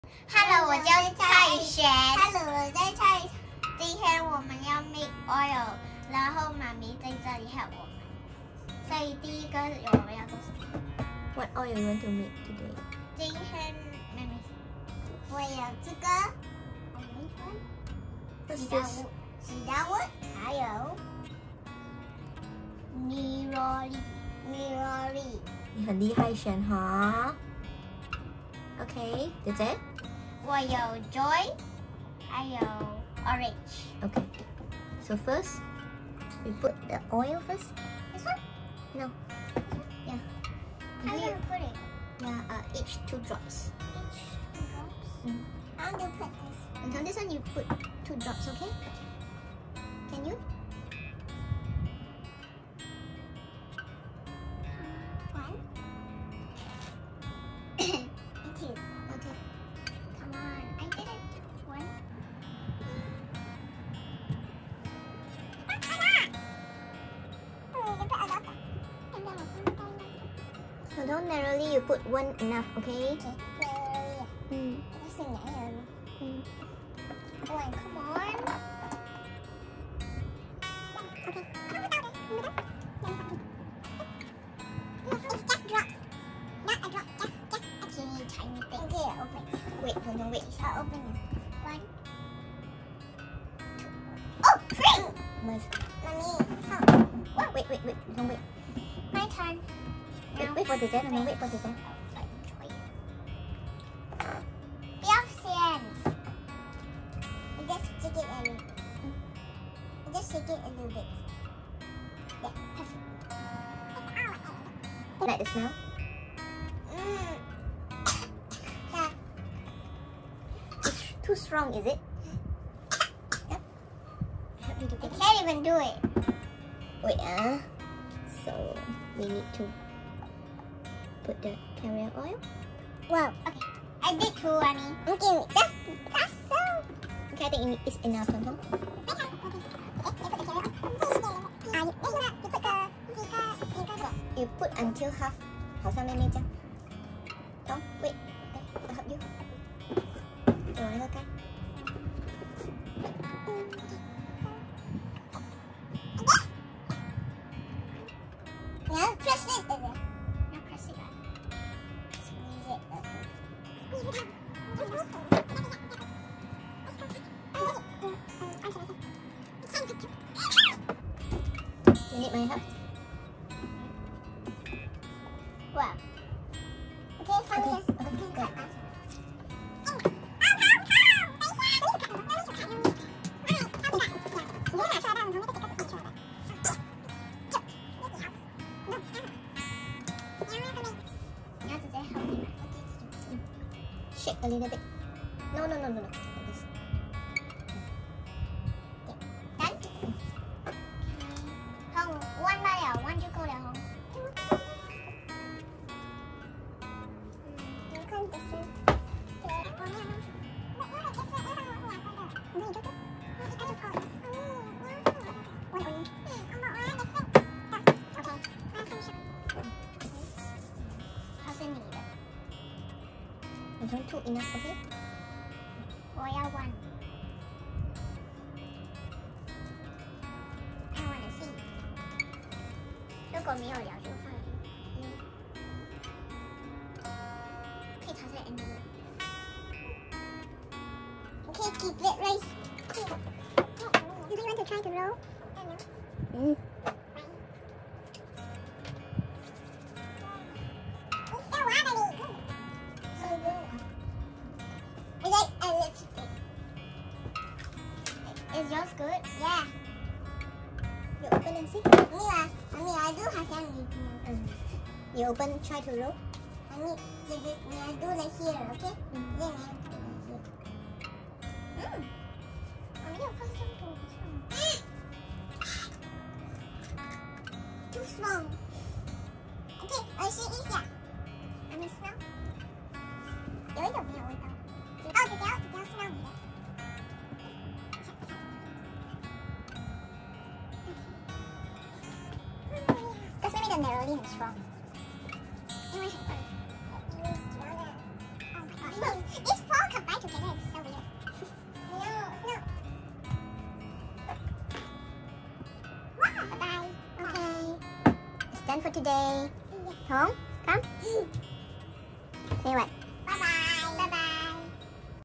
【玹彤DIY】自己DIY专属精油香水roll on
每天看妈妈涂香香,我们跟妈妈要求要来做属于自己的香水roll on,我们也要每天涂香香! 原来那么容易做而已, 你也来做看看吧!
姐姐选了Joy + Orange
妹妹选了Cedarwood + Neroli
这两个组合都好香啊~~